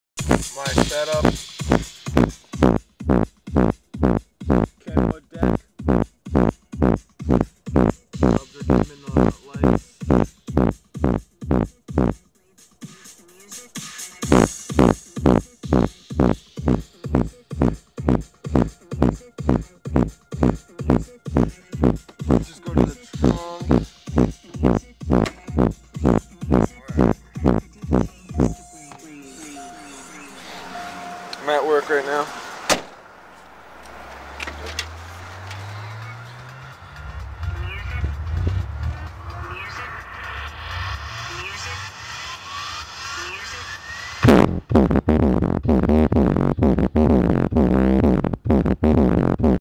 New sound system in my cougar
2 magnum 10 inch with a rockford fosgate amp